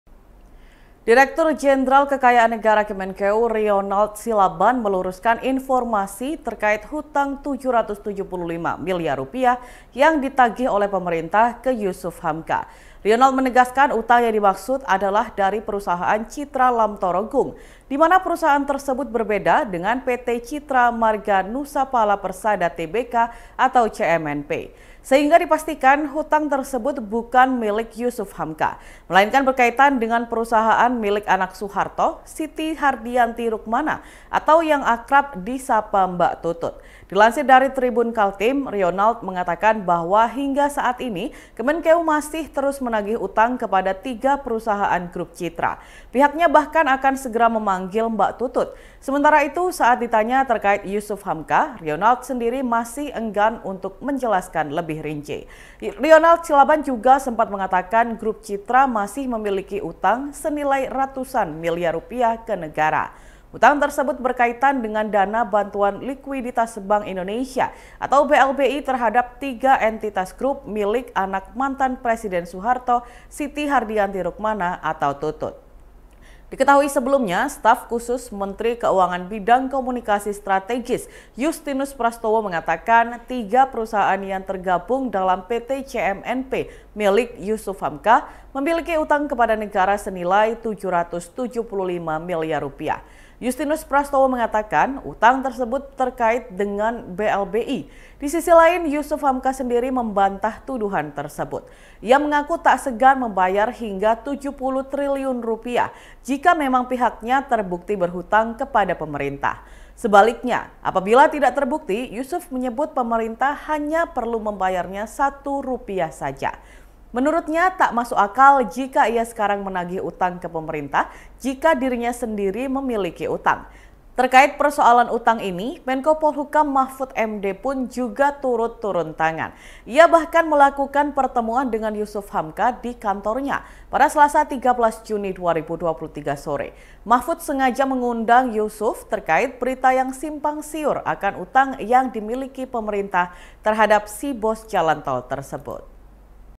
UPDATE Kemenkeu Klarifikasi Hutang Rp 775 Miliar: Bukan Jusuf Hamka tapi Perusahaan Tutut
TRIBUNMADURA.COM - Direktur Jenderal Kekayaan Negara Kemenkeu, Rionald Silaban, meluruskan informasi terkait hutang Rp 775 miliar yang ditagih oleh pemerintah ke Jusuf Hamka.

Rionald menegaskan, utang yang dimaksud adalah dari perusahaan Citra Lamtoro Gung.

Di mana perusahaan tersebut berbeda dengan PT Citra Marga Nusaphala Persada Tbk (CMNP).

Sehingga dipastikan, hutang tersebut bukan milik Jusuf Hamka.